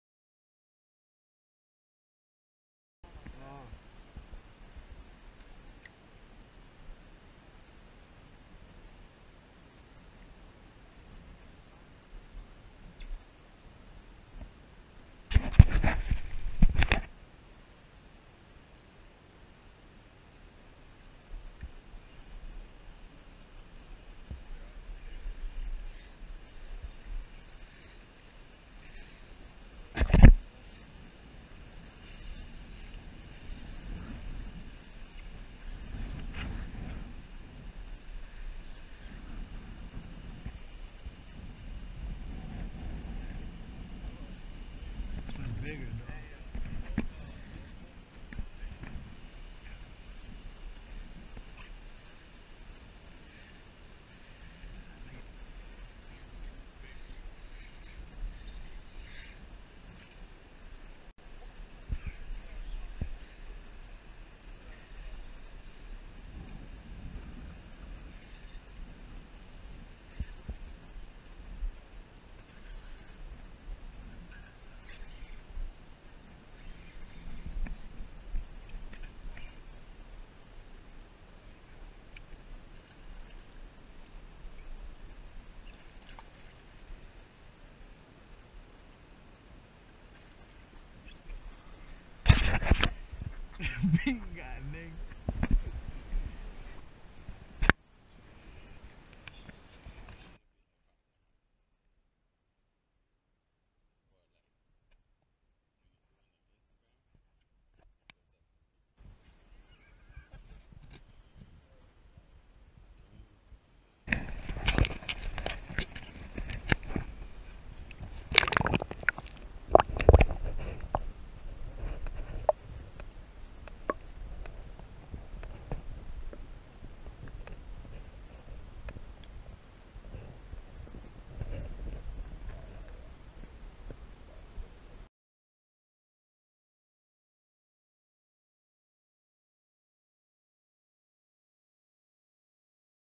PEACOCK STUD!! MUST SEE
Went out with my brother Capt. Drew to slay some peacock. check him out on Instagram @drews_bait_shack  ...